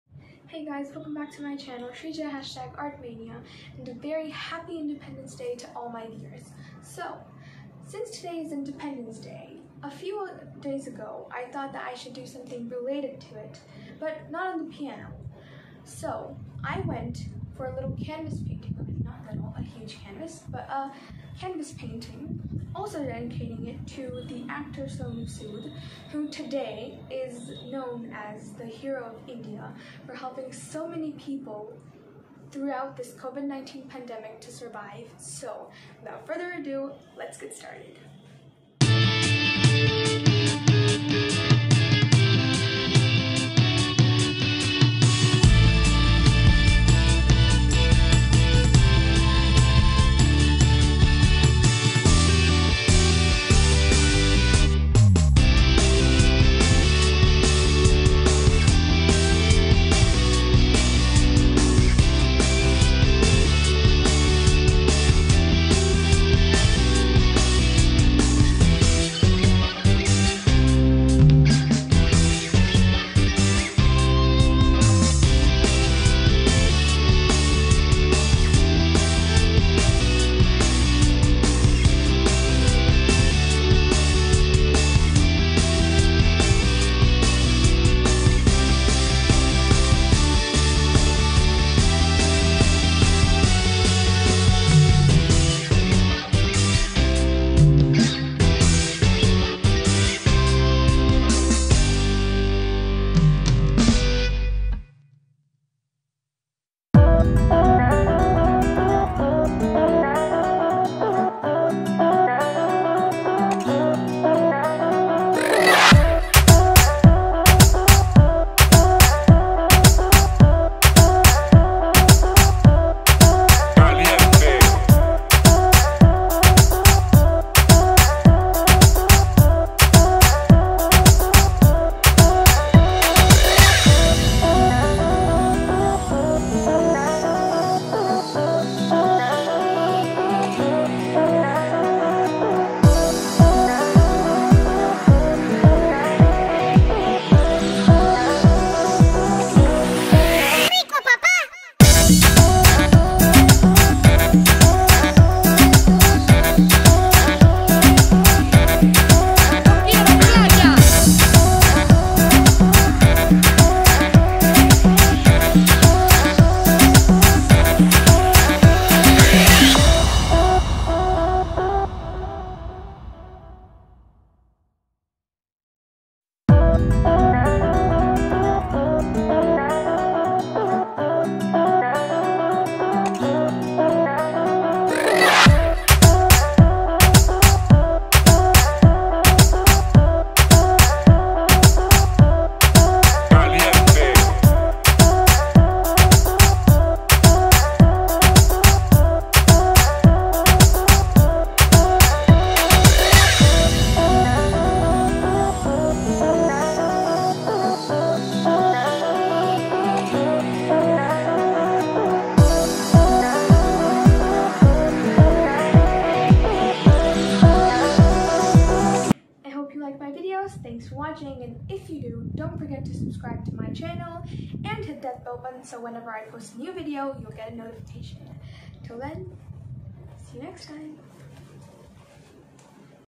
Independence Day inspired painting on canvas 🇮🇳 Shreeja# Artmania
Hey guys ! Welcome back and a very warm Independence Day to all the Indian citizens! In today’s video I uploaded a time lapse of my recent drawing, you can see the entire picture in my Instagram page

Instagram account name- Shreeja_001